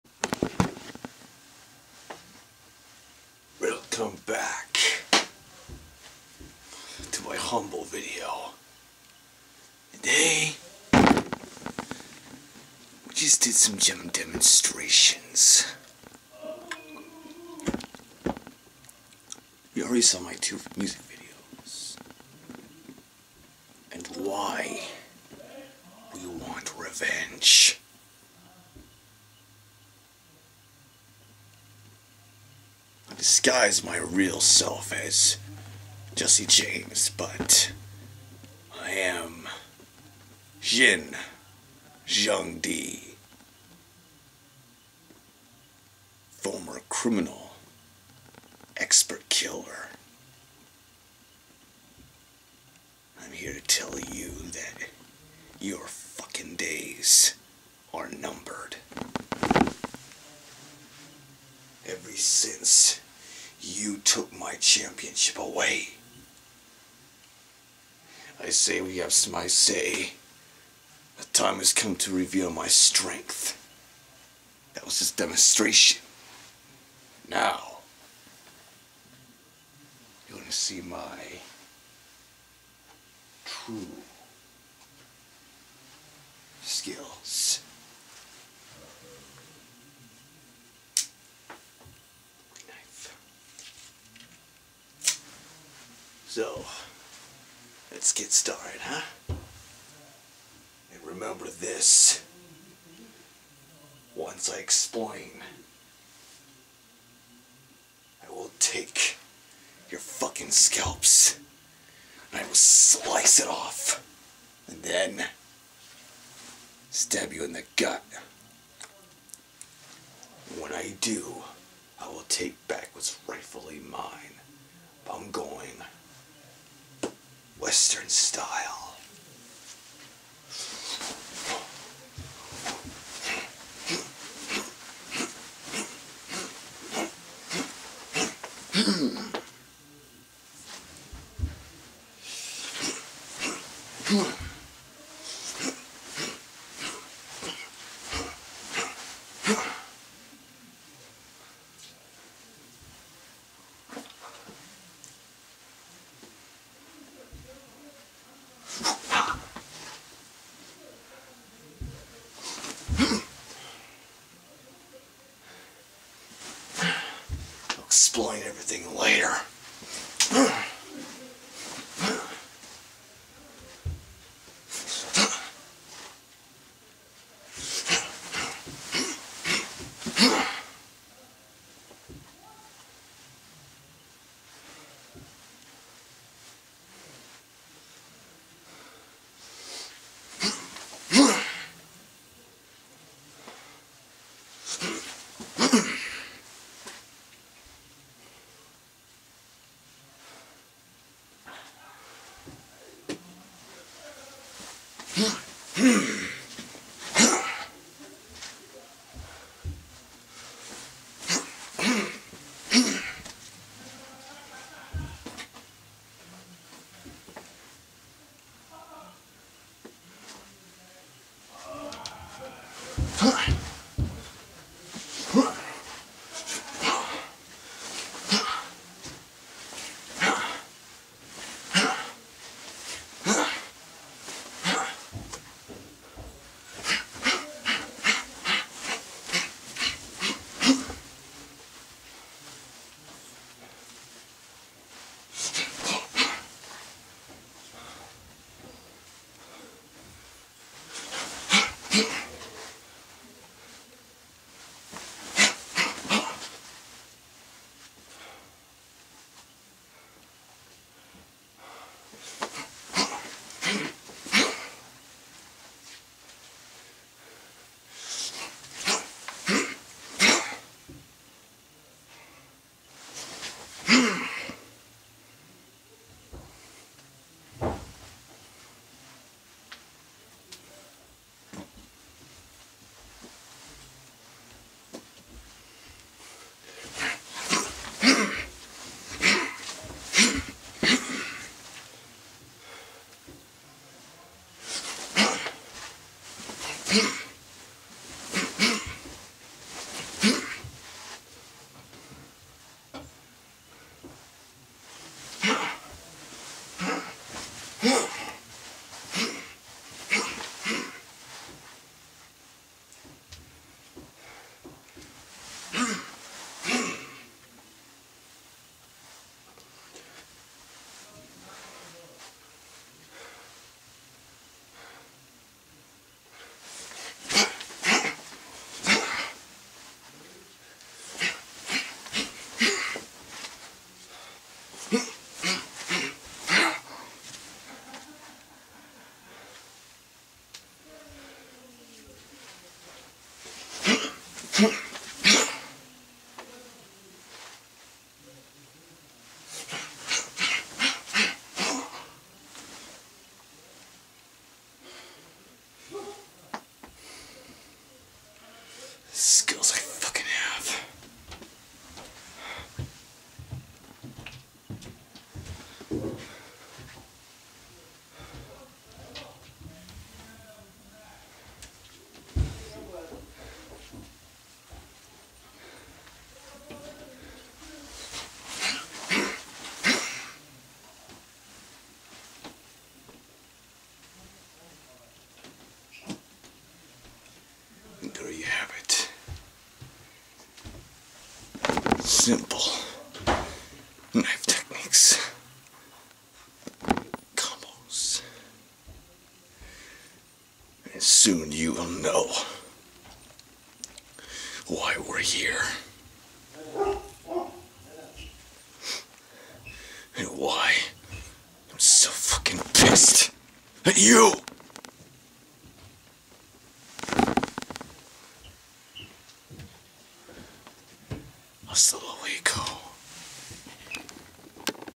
hellunleashed part 1
evile reaper makes his plans known to all of his enemies that he is coming for them soon to exact revenge upon them who despied him all these mouths